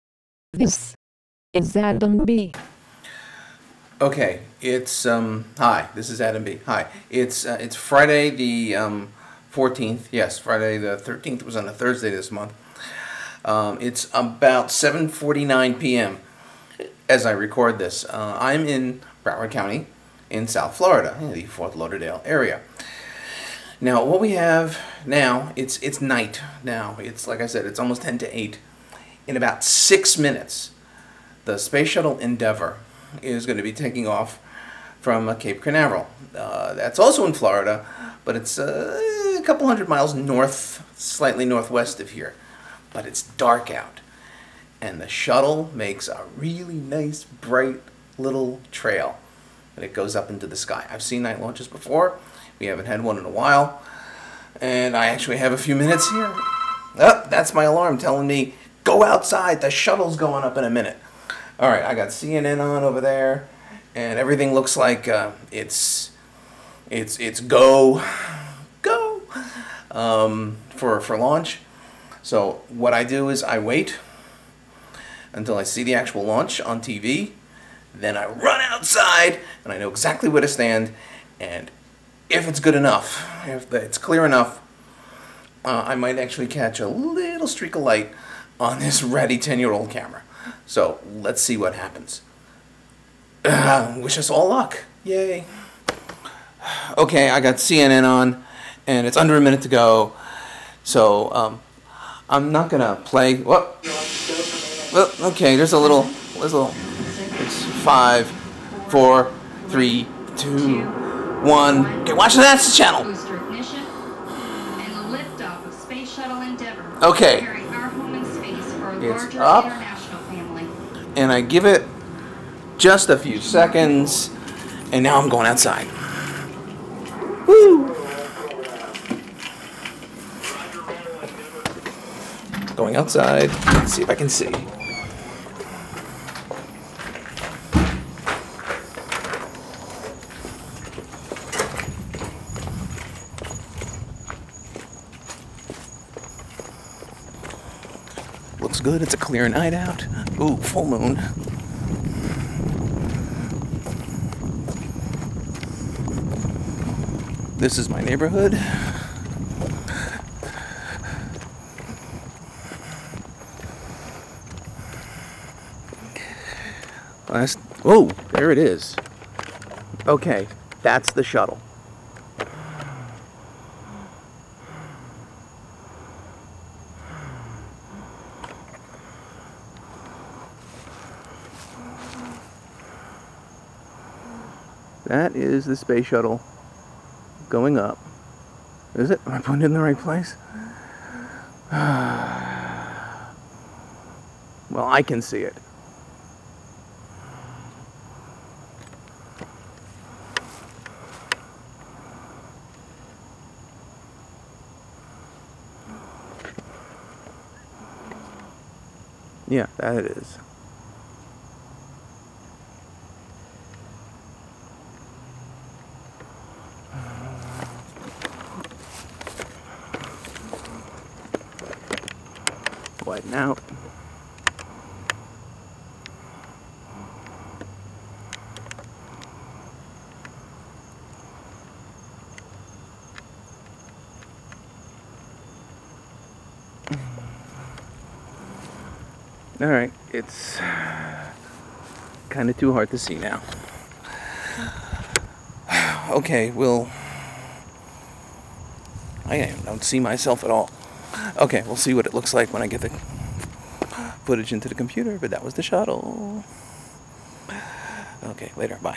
Lift-off!
I take my camera outside to get a (distant) look at the launch of the Space Shuttle on the evening of 11/14/2008.